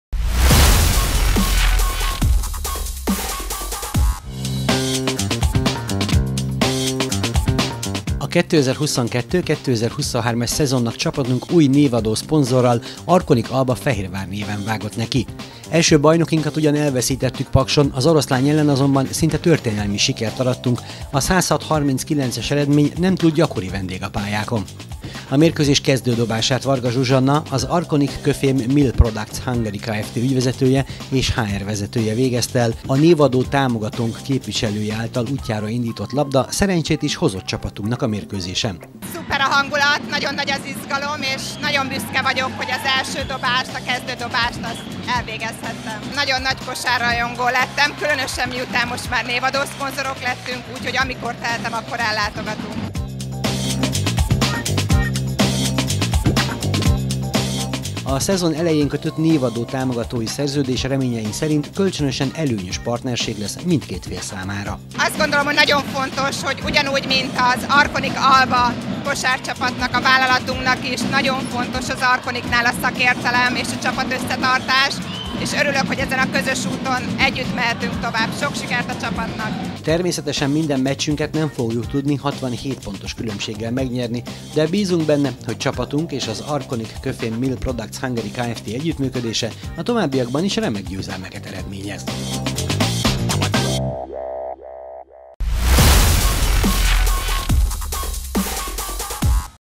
Névadó támogatónk ügyvezetője indította útjára a labdát
Az MVM-OSE Lions elleni mérkőzés kezdődobását Varga Zsuzsanna, az Arconic – Köfém Mill Products Hungary Kft ügyvezetője és HR vezetője végezte el.